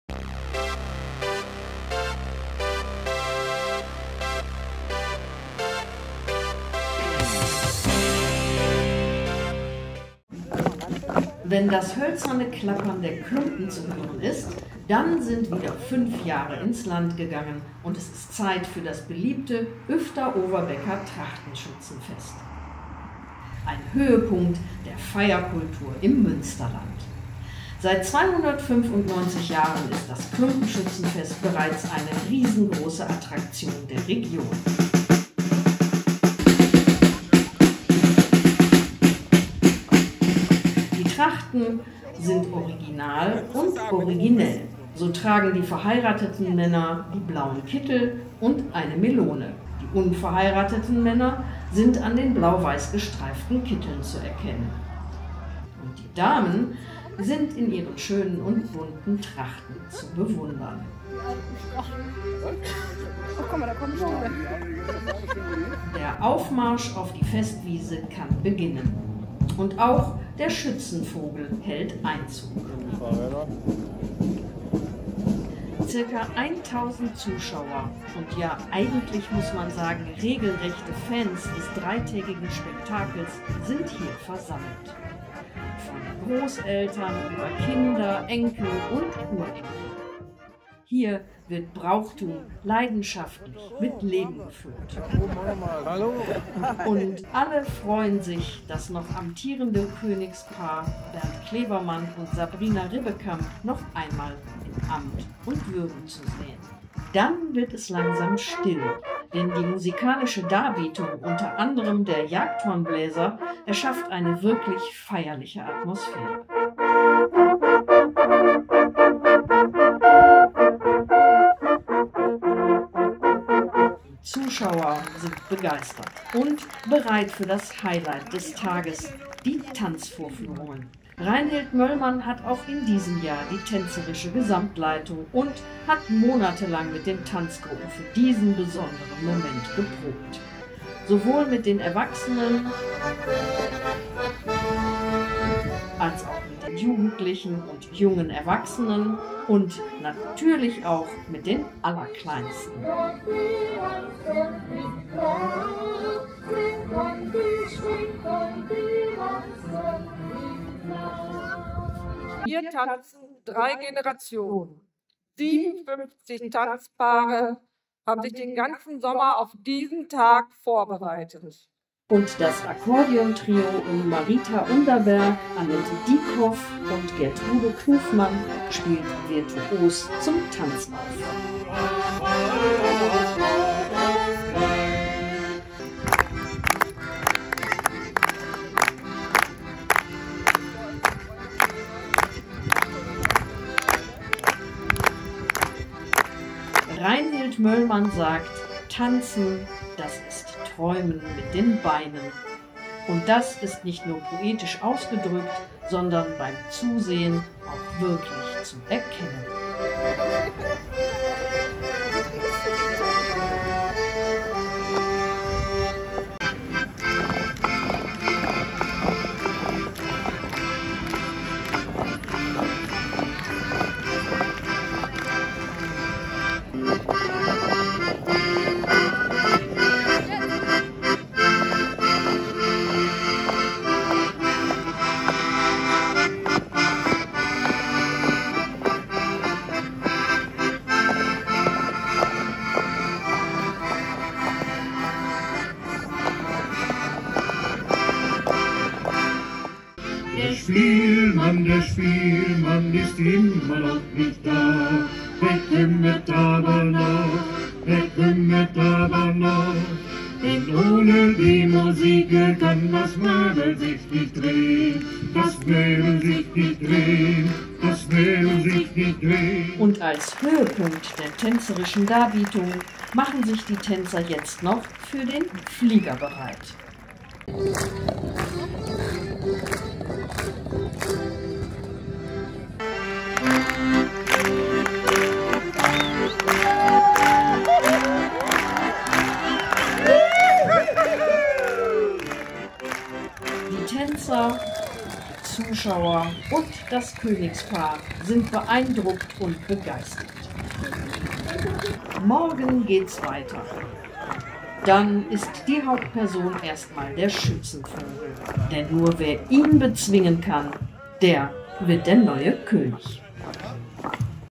Trachtenschützenfest Uefte-Overbeck 2024
Alle fünf Jahre wird in Uefte-Overbeck das Trachtenschützenfest gefeiert.